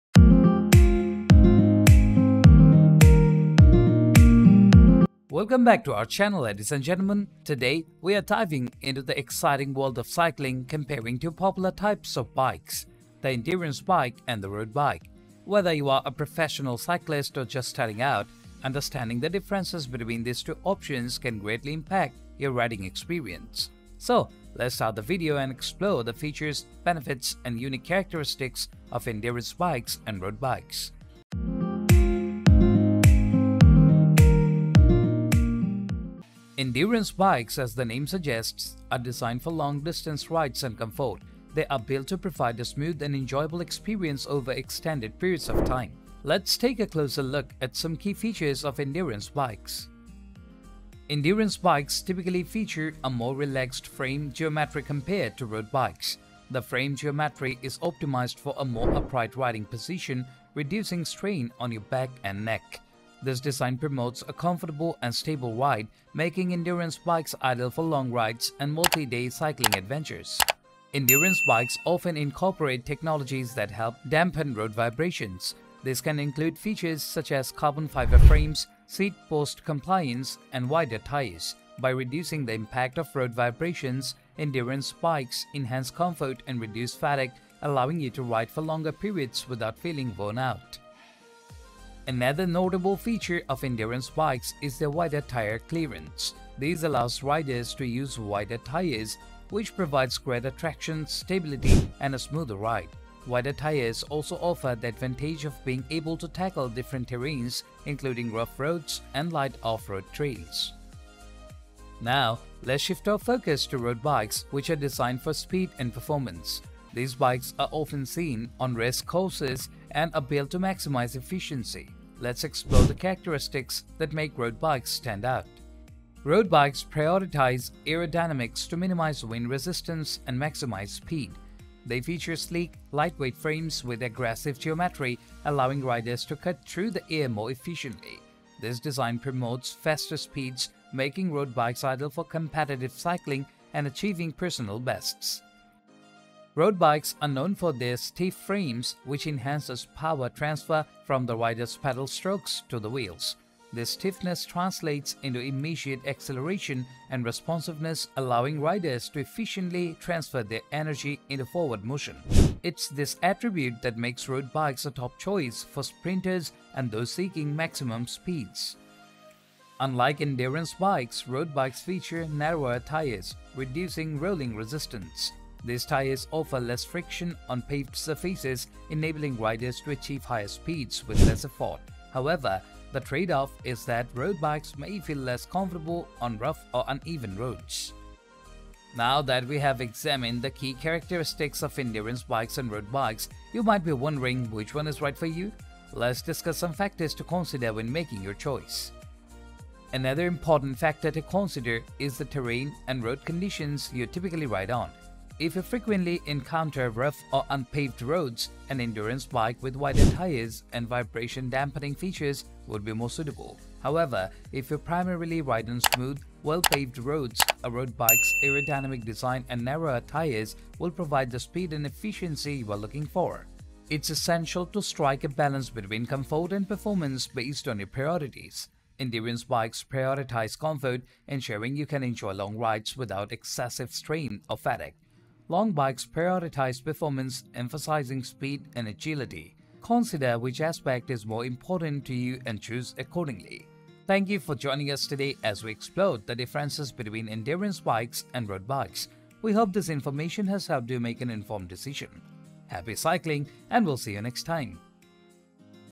Endurance Bike vs Road Bike - What's The Difference?
Discover the key differences between endurance bikes and road bikes to help you make the right choice for your cycling needs. We break down the essential factors to consider, from comfort to speed, so you can ride with confidence.